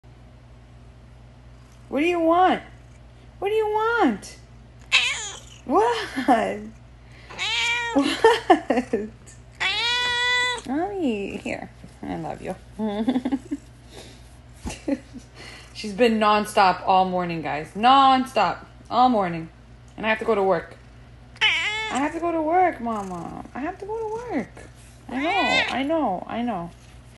Lillac Point Nola Not Letting Me Go To Work
Nola - Our beautiful little Lillac Point Siamese Cat is very very affectionate!